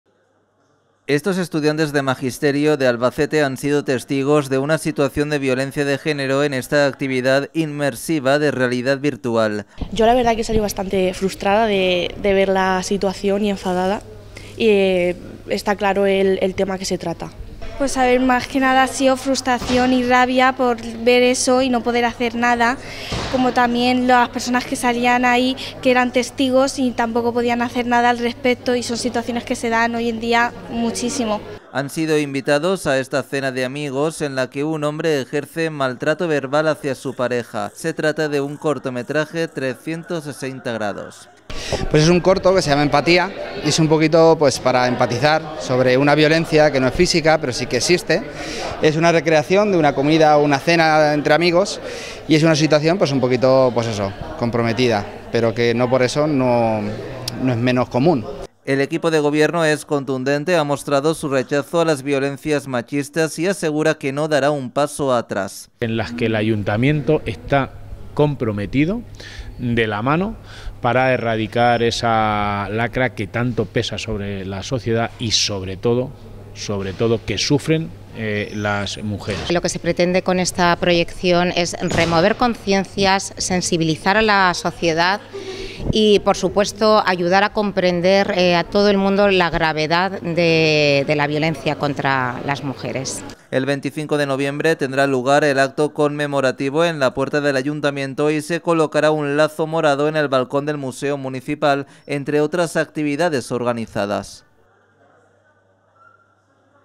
Realidad virtual para concienciar sobre la violencia de género
Estudiantes de Magisterio han sido invitados a una cena de amigos en la que han experimentado una situación de violencia de género. Se trata de una actividad inmersiva, gracias a la realidad virtual, para concienciar sobre esta lacra. El equipo de Gobierno se ha mostrado contundente, mostrando su rechazo a las violencias machistas y asegurando que no dará un paso atrás. El 25 de noviembre tendrá el acto conmemorativo en la puerta del Ayuntamiento.